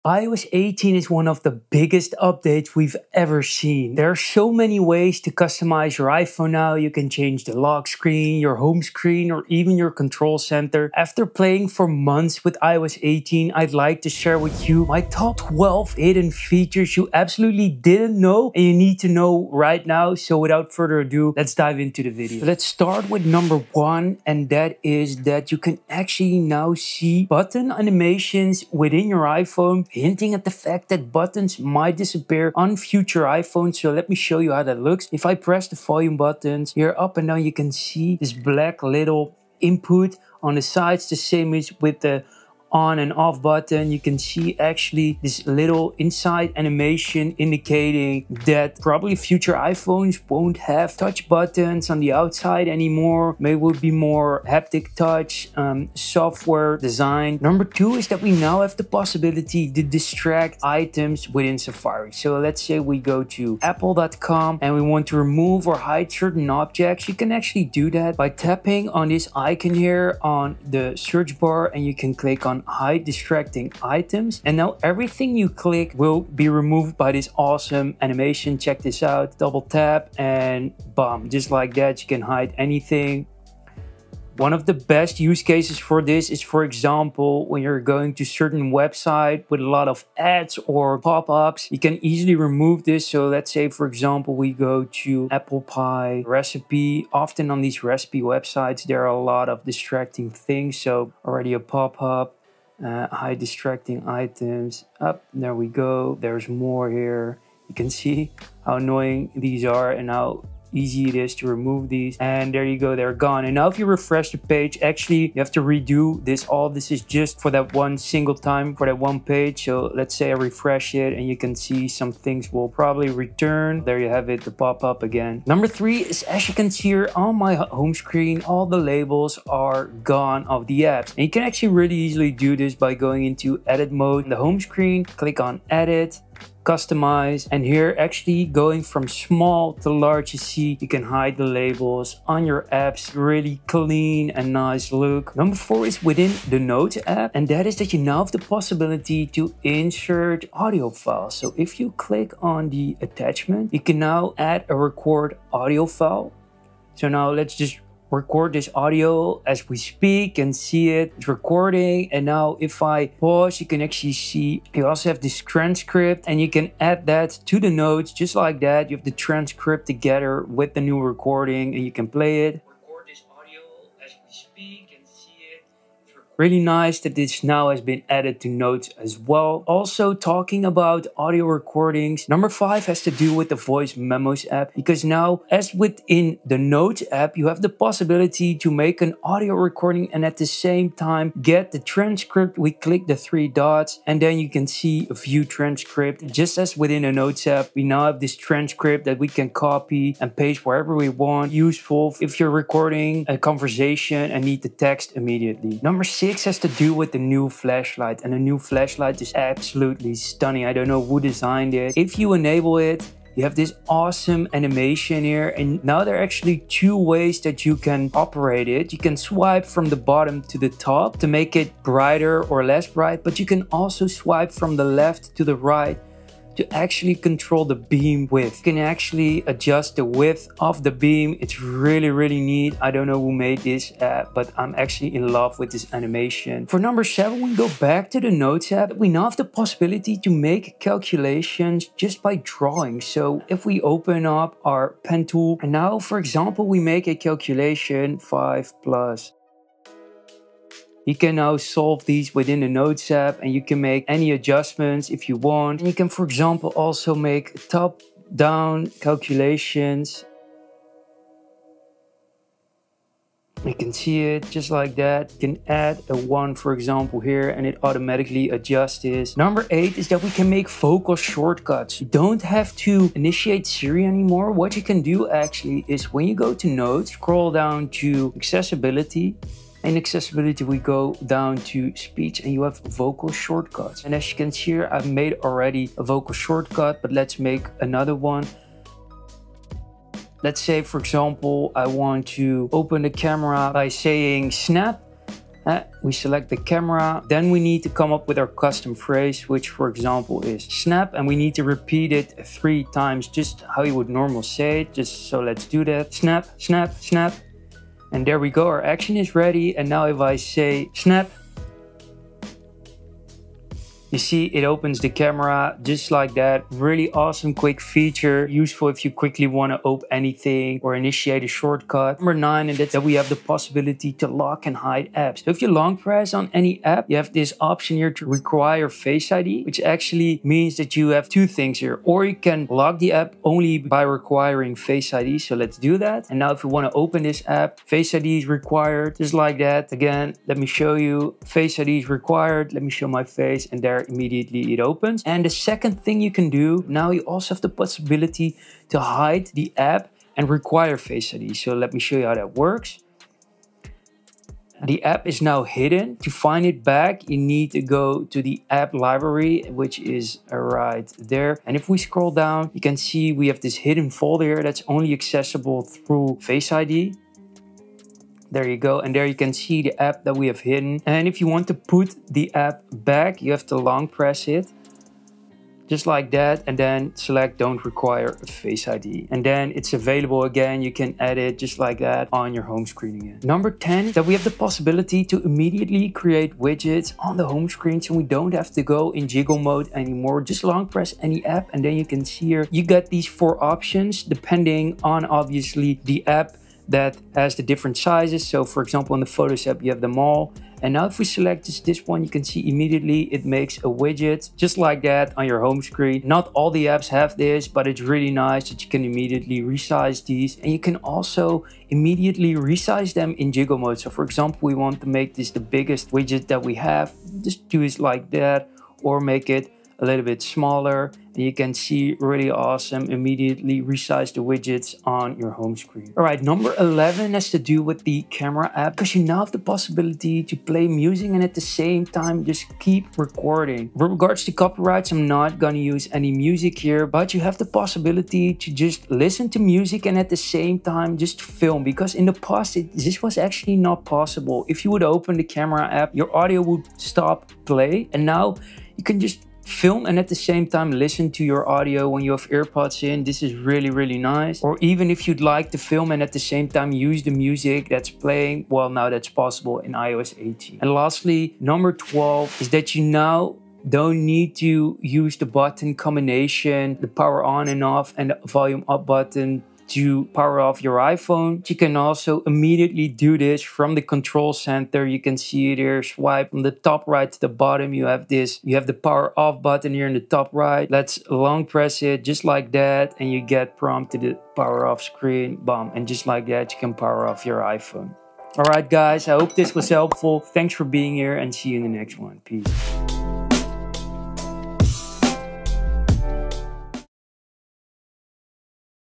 12 Hidden iOS 18 Features You didn't know yet!
As with every iOS update there are a couple hidden features not many people actually know. Personally I love to discover these hidden features when a new iOS update is released.
These aren't your typical iOS 18 Features, no these iOS 18 Features are a bit more advanced.
Often we miss a lot of the iOS 18 Features so let me show you my Top 12 Hidden iOS 18 you probably haven't seen yet. For example did you know that you can now turn off your iPhone with the help of the new iOS 18 Control Center?

⏱ Timestamps:
00:00 Introduction
00:23 New Button animations
00:57 Distract items in Safari
02:05 Delete App Labels
02:26 Insert Audio files in Notes App
03:02 Transcript Audio recording
03:26 iOS 18 iPhone Flashlight
03:57 Make calculations in Notes
04:29 Make Vocal Shortcuts
05:23 Lock & Hide Apps
06:24 Create Widgets on Home Screen
07:06 Play Music while recording
07:41 iPhone Power on/off button
08:10 Outro

▶Follow our profound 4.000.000+ people community
AppleDsign: